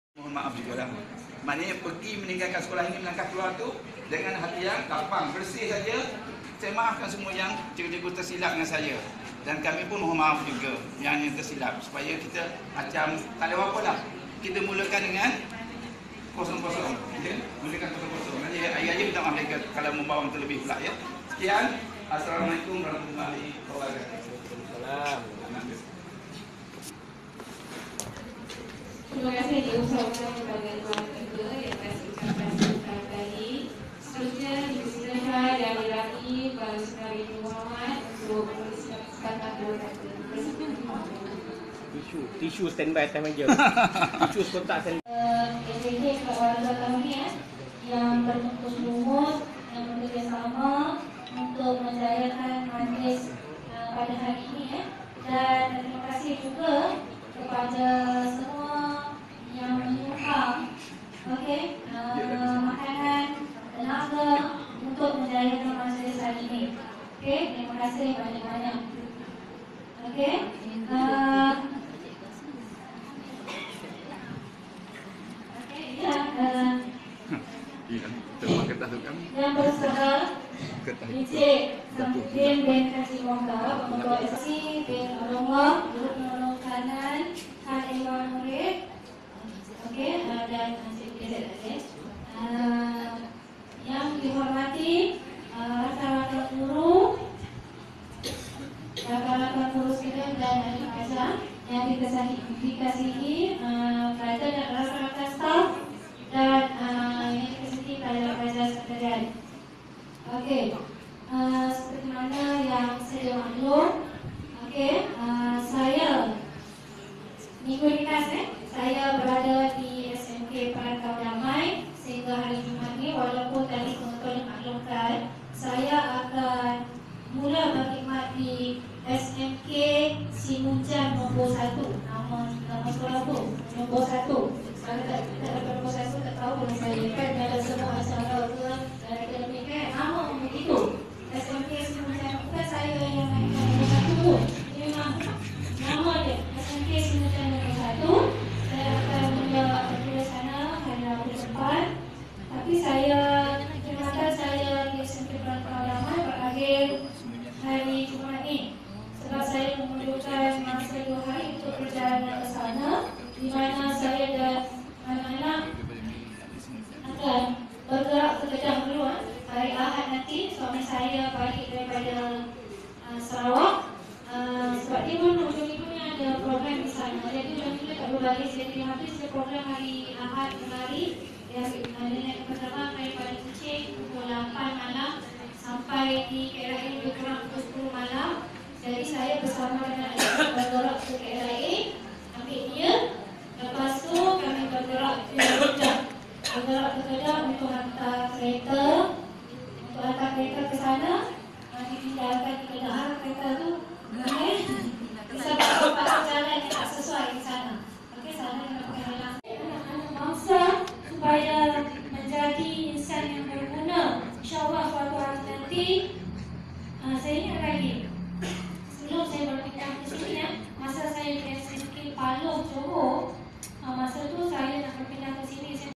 2019 MAJLIS PERPISAHAN PN. RUSLINA MOHAMAD
RABU, 9 OKT 2019. BERPINDAH KE SMK SIMUNJAN NO 1